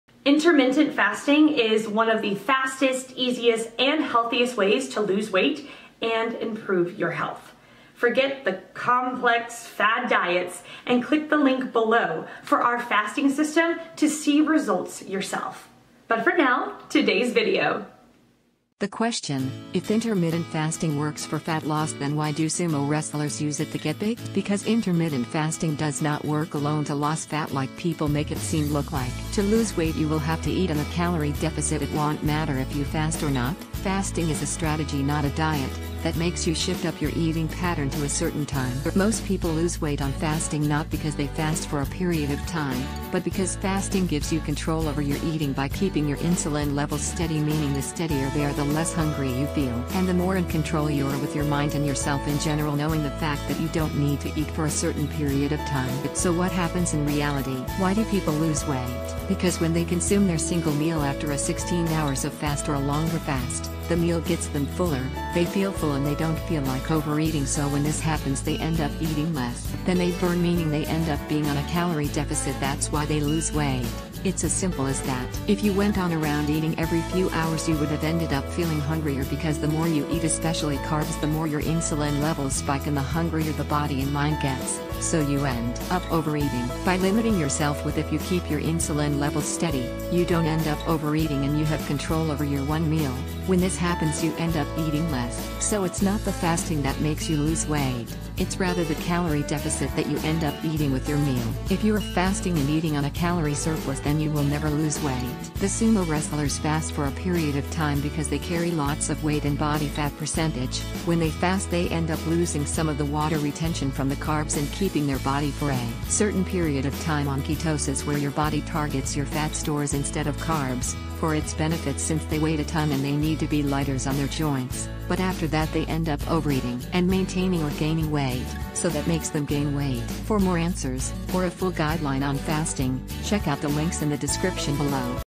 If Intermittent Fasting Works For Fat Loss Then Why Do Sumo Wrestlers Use It To Get Big?
Most people lose weight on fasting not because they fast for a period of time, but because fasting gives you control over your eating by keeping your insulin levels steady meaning the steadier they are the less hungry you feel